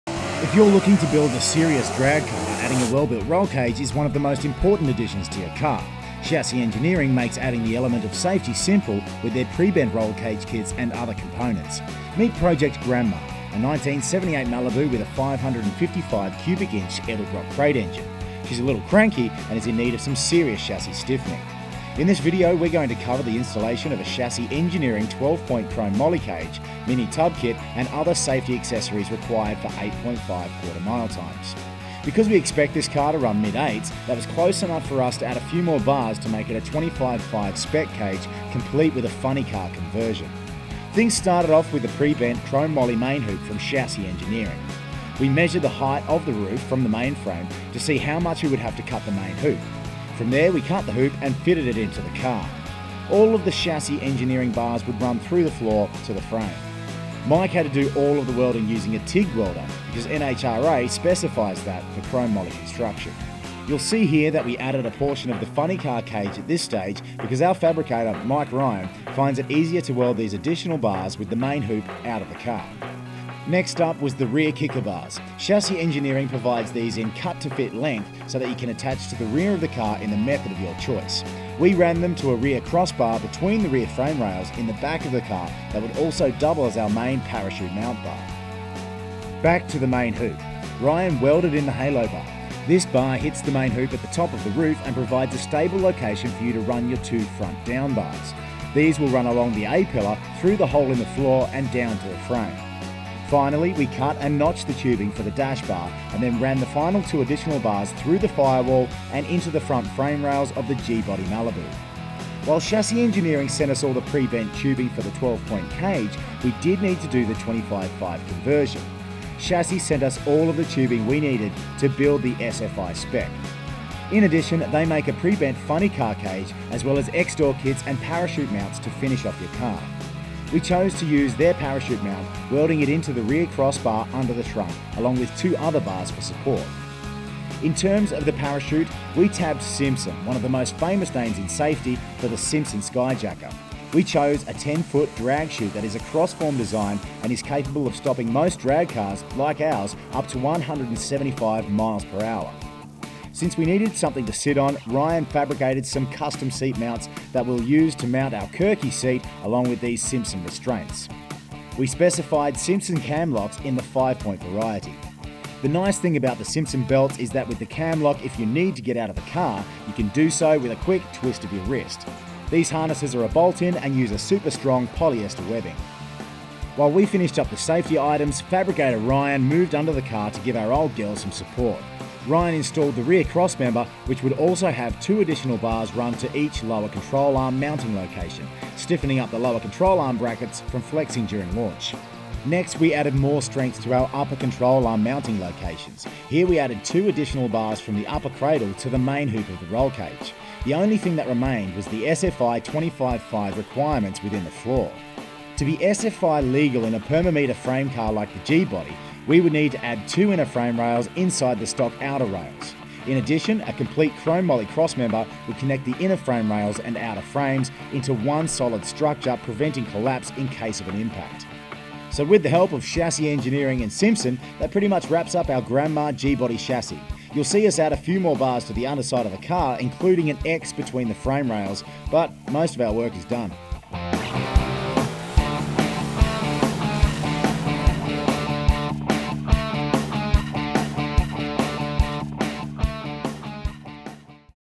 We install a Chassis Engineering 25.5 Cage in our 78 Malibu
Using a complete roll cage kit from Chassis Engineering, we got our 78 Malibu, Project Grandma, ready for low 8 second quarter miles.